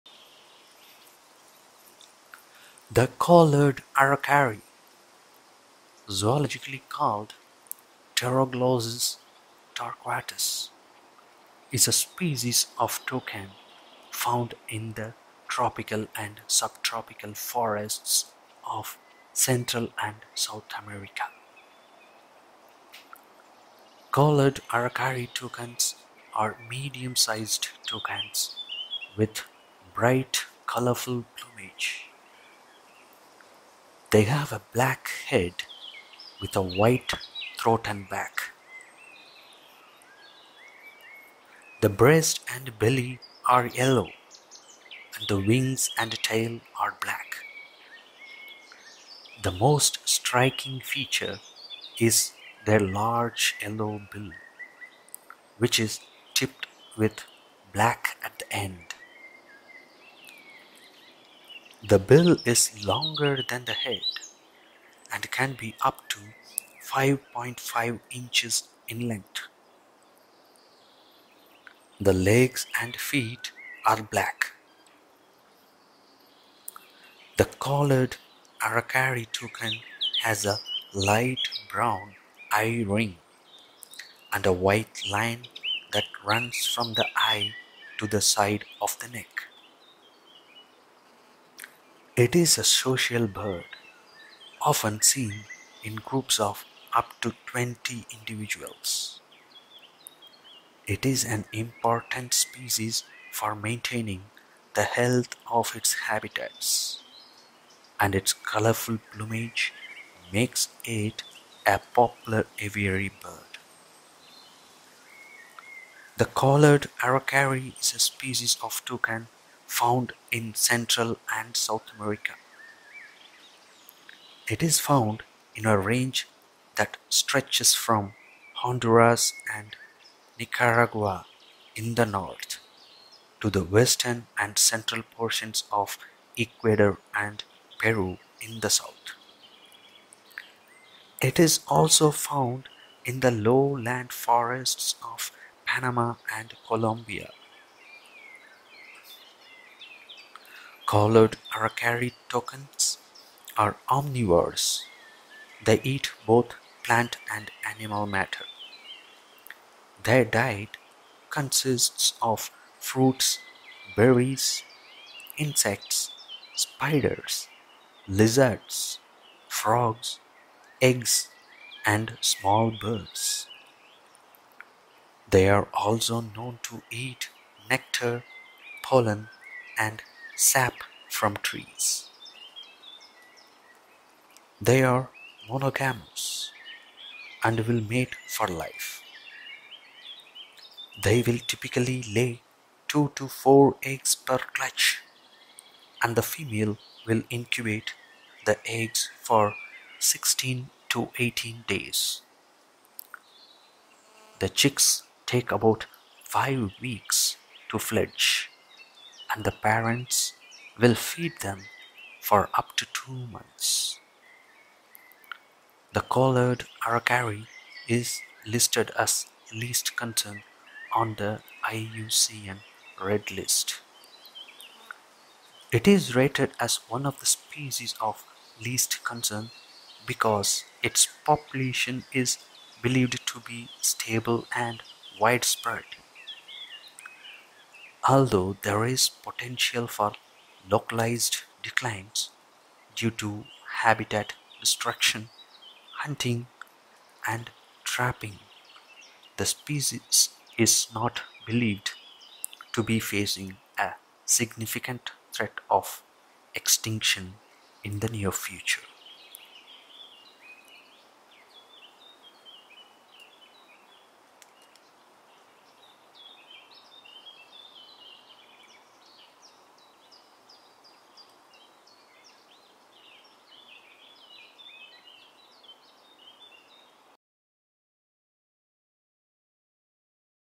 Collared Aracari Toucan - Get to Know the Costa Rica Bird Aracari Toucan
The Collared Aracari Toucan is a beautiful and colorful tropical bird native to Central and South America. This species of toucan is characterized by its bright yellow and black collar around its neck, which gives the bird its name. The Collared Aracari Toucan has a medium-sized body, with a short and slightly curved bill, and a long tail. This species is often seen in pairs or small flocks, and its loud, high-pitched call can be heard from long distances away. The Collared Aracari Toucan is a popular aviary bird, and can make an entertaining and lively pet.